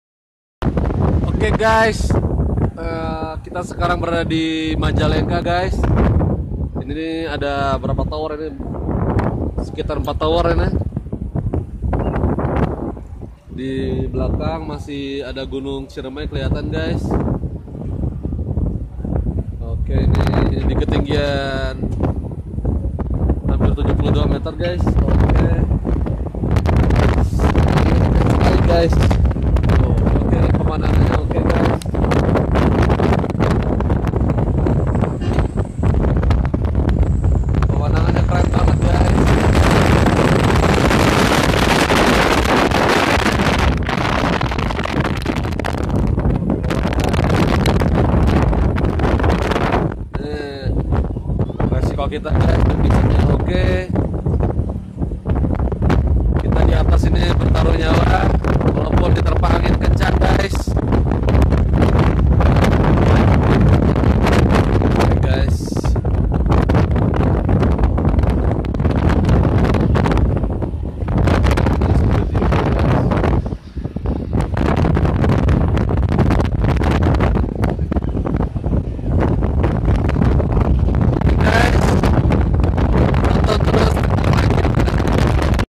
INDONESIA TOWER-RED BULL XTREME-BELIVE IT OR NOT!!TOWER RANGER/MENARA PUNCAK RIGGER BERAKSI
Tower ranger beraksi demi sinyal oke